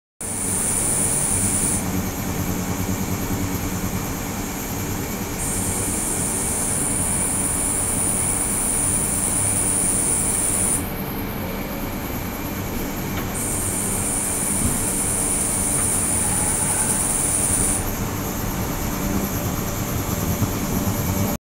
３種類の異なる周波数の「超音波振動子」を利用する技術 ultrasonic-labo
３種類複数の異なる周波数の「超音波振動子」を利用して

　１）パワー洗浄シリーズ（28KHz　300W）
　２）パワー洗浄シリーズ（40KHz　50W）
　３）精密洗浄シリーズ（72KHz　300W）

３種類の異なる周波数の「超音波振動子」を利用する技術

２種類の異なる「超音波振動子」を同時に照射するシステム